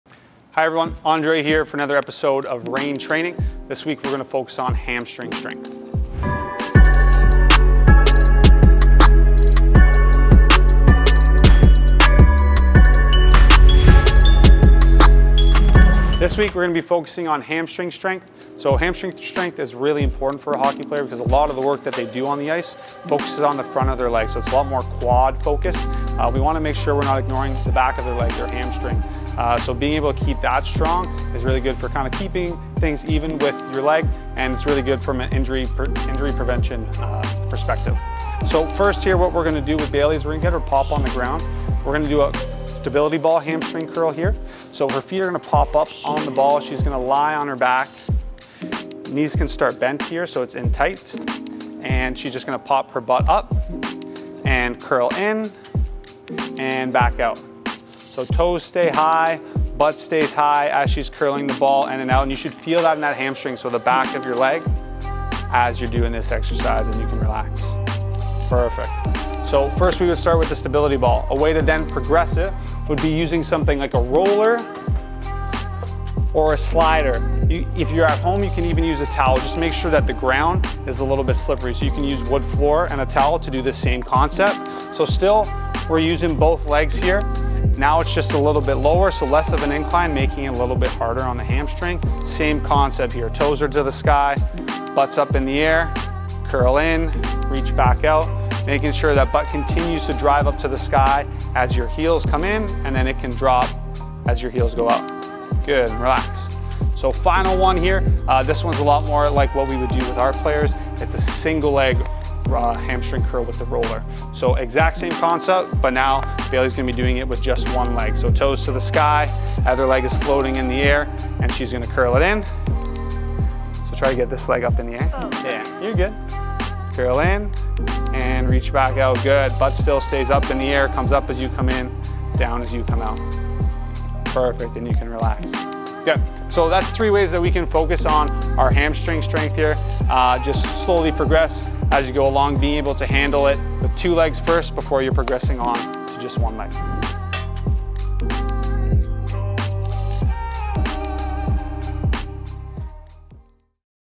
Episode 5 - Hamstring Strength
We’re back for another episode of Reign Training! This week, Coach Carere is hittin’ the hammys!